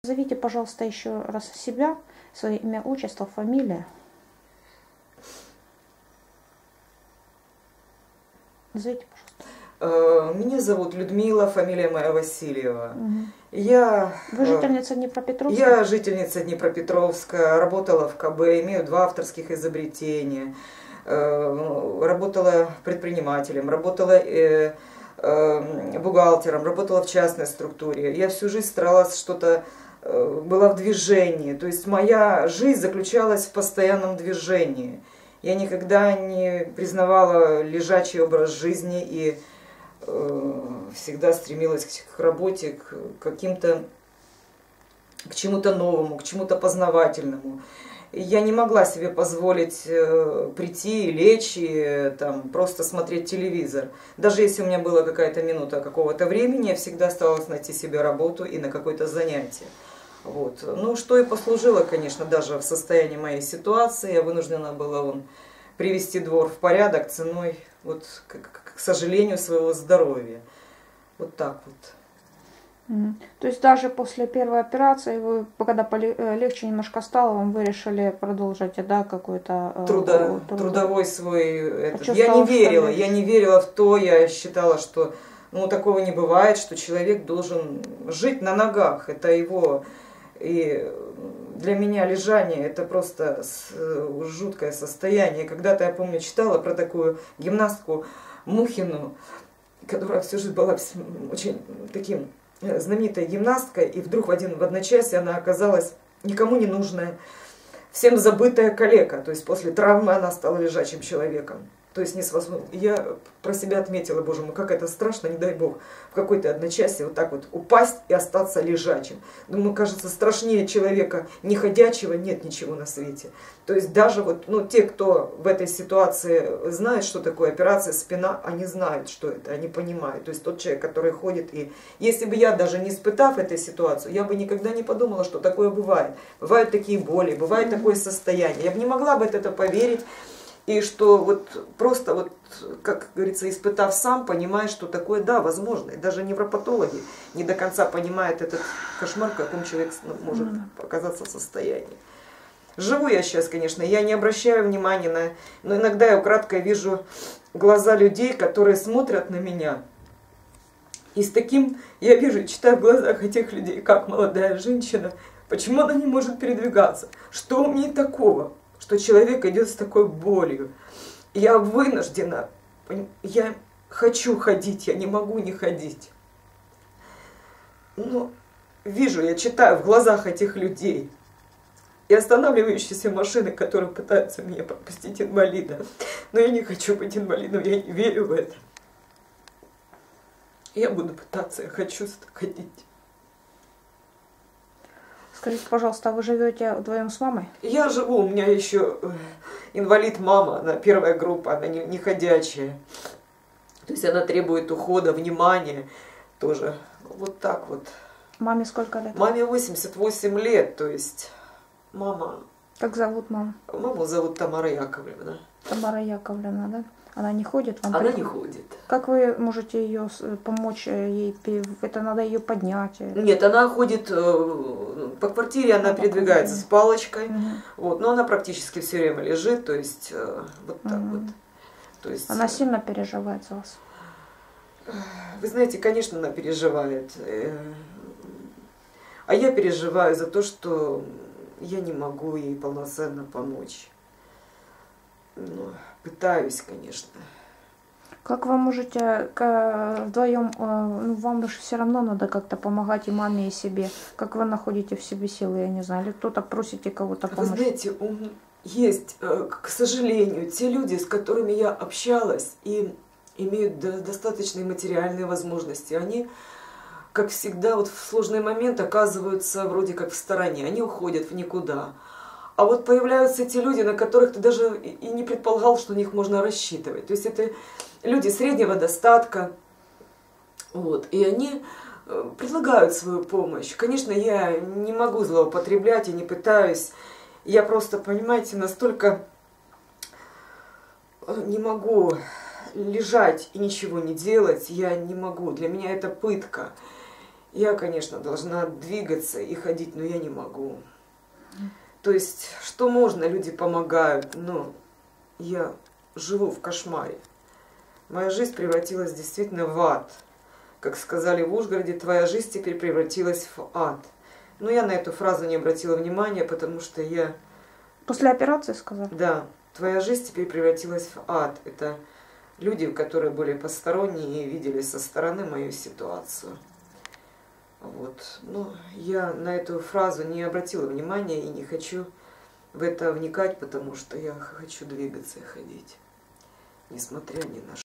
СПАСИТЕ ЖЕНЩИНУ. ЛЮДМИЛА ВАСИЛЬЕВА ПРОСИТ О ПОМОЩИ
История, после которой не заснуть.Жительница Днепропетровска, Людмила Васильева, стала инвалидом из-за врачебной халатности. Женщина терпит ужасающие боли в позвонке и ноге, практически не может ходить.  Людмила Алексеевна молит о помощи всех небезразличных людей. Врачи немецкой клиники готовы помочь. Но у украинки, которая из-за халатности украинских медиков стала инвалидом- нет денег. Все обращения женщины к светилам медицины Украины и властям - не услышаны.   В родной стране до беды этого человека  будто никому нет дела. И все же мы просим помочь этой женщине! Она ведь  не суда виновных желает, просто хочет жить и не испытывать боли.
Васильева Людмила  Алексеевна (1963Г.Р)
моб. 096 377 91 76 ПОЗВОНИТЕ ЕЙ ! ПОМОГИТЕ!
Приватбанк.
КАРТА МАСТЕР КАРТ ГОЛД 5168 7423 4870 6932